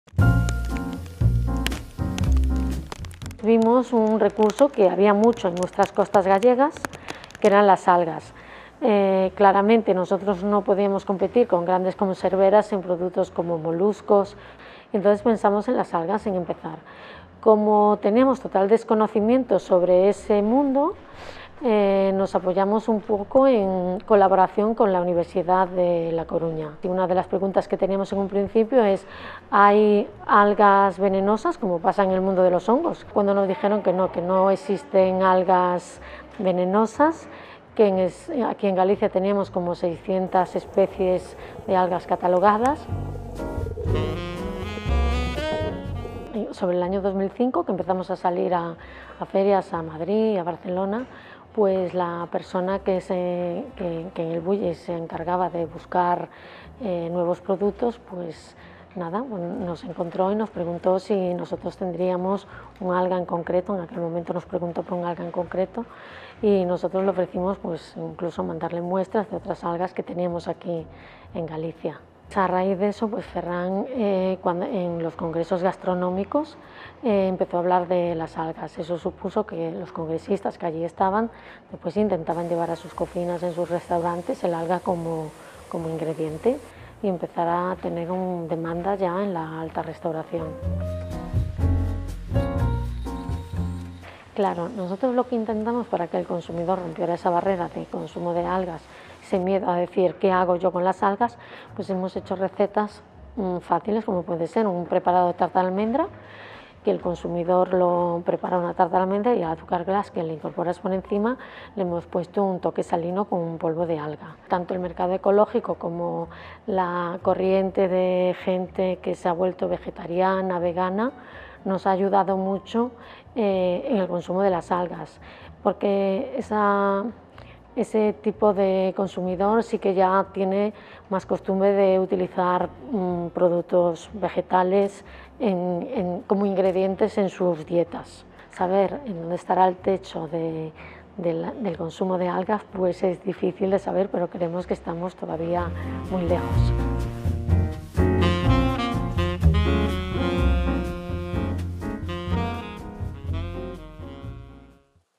La huerta submarina de Rosa Mirás | Personaje | El País Semanal
Hace 20 años apostó por comercializar en Galicia algas para el consumo y fundó junto a su marido Porto-Muiños. El auge de la alta cocina, la búsqueda de un estilo de vida más saludable y las dietas vegetarianas han contribuido a impulsar un negocio que tiene a Ferran Adrià como padrino.